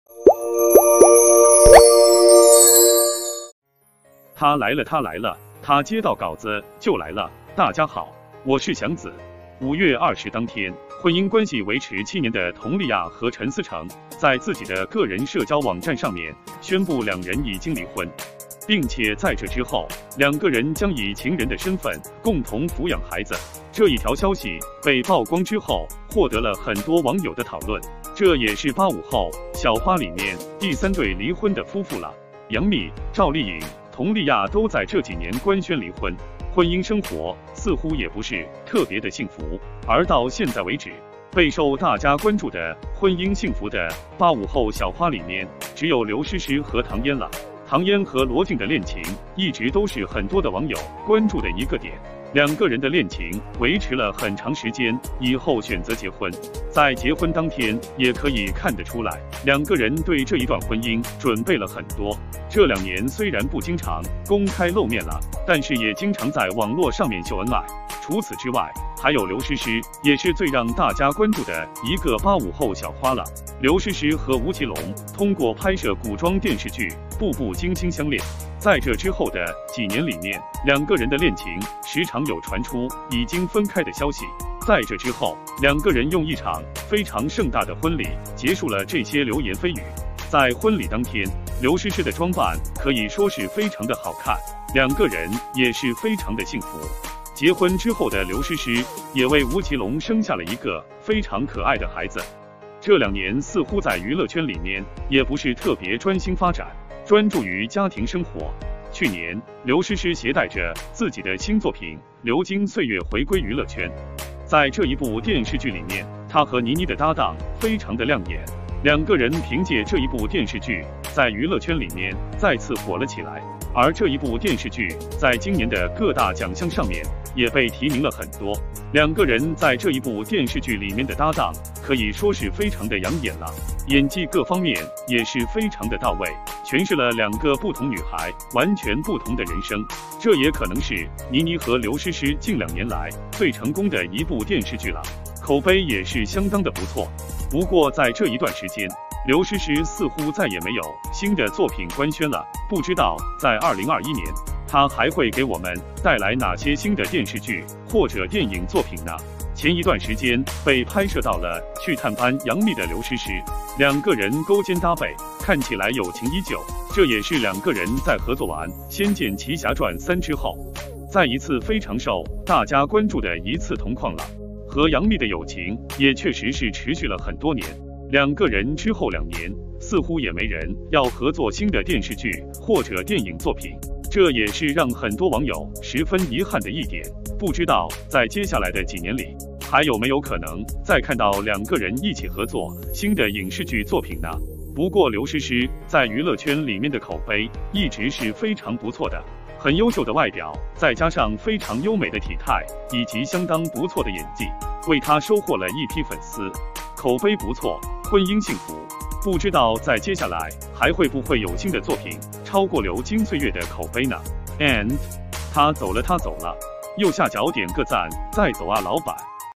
刘诗诗婚姻美满，但他的资源却远不及同期。会有超越黄金时代的作品吗
五月二十当天，婚姻关系维持七年的佟丽娅和陈思诚在自己的个人社交网站上面宣布两人已经离婚，并且在这之后，两个人将以情人的身份共同抚养孩子。